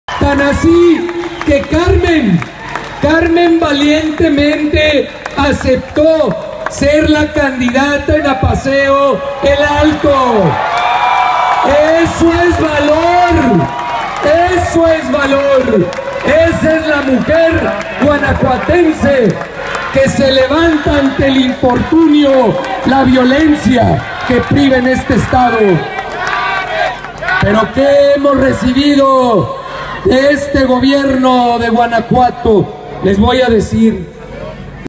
R. Sheffield oficializa que María del Carmen Ortiz tomará el lugar de su esposo, Remedios Sánchez
Llegan simpatizantes de Apaseo el Alto a evento de AMLO en Celaya; viuda del candidato a alcalde por Morena, lo sustituirá en la contienda.